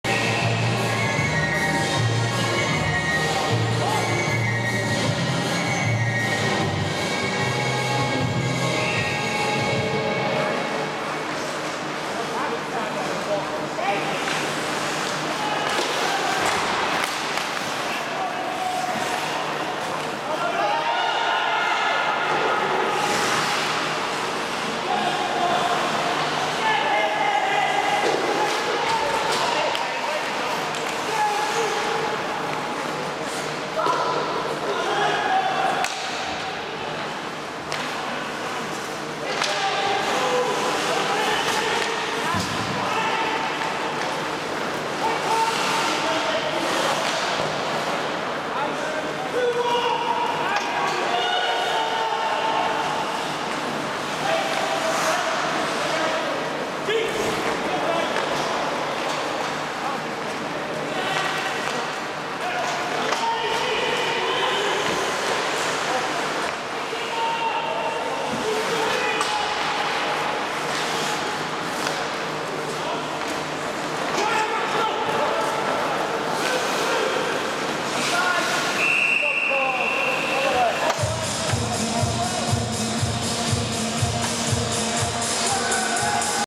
20131026 Manchester vs Sheffield NIHL 0014
Manchester Minotaurs vs. Sheffield Spartans NIHL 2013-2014 Ice Hockey League Game Altrincham Greater Manchester England Canon Legria HF S21